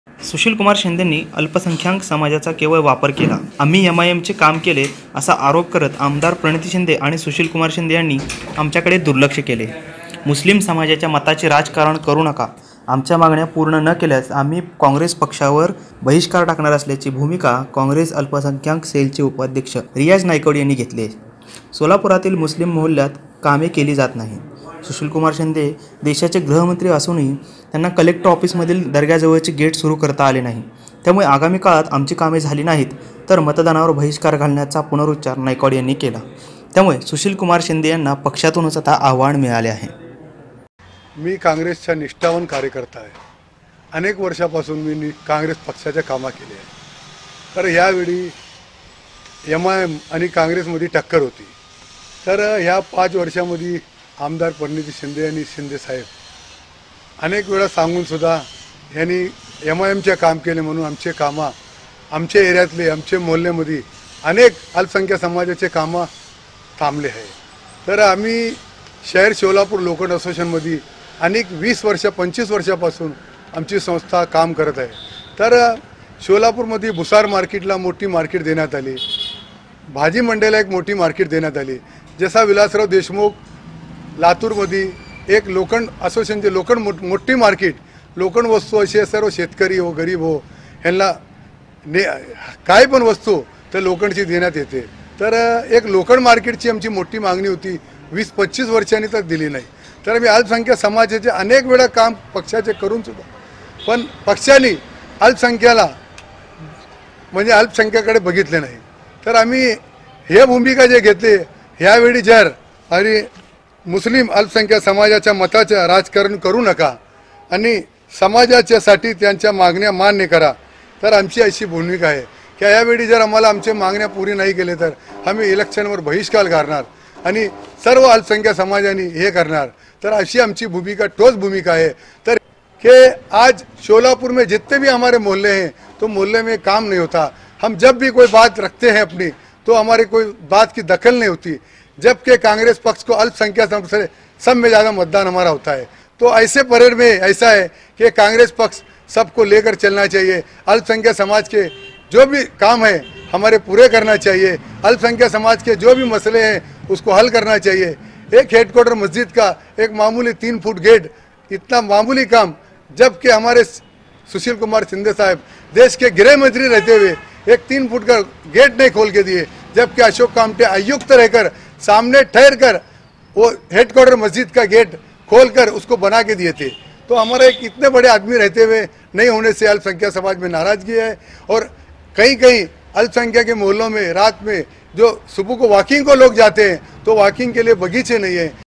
Solapur : ...तर मुस्लिम समाज कॉंग्रेसला मतदान करणार नाही : कॉंग्रेस पदाधिकारी
सोलापूर : सुशीलकुमार शिंदेंनी अल्पसंख्याक समाजाचा केवळ वापर केला. आम्ही एमआयएमचे काम केले असा आरोप करत आमदार प्रणिती शिंदे आणि सुशीलकुमार शिंदे यांनी आमच्याकडे दुर्लक्ष केलेय. मुस्लिम समाजाच्या मताचे राजकारण करु नका. आमच्या मगण्या पुर्ण न केल्यास आम्ही कॉंग्रेस पक्षावर बहिष्कार टाकणार असल्याची भूमिका कॉंग्रेस अल्पसंख्यांक सेलचे उपाध्यक्ष रियाज नाईकवाडी यांनी घेतलीय.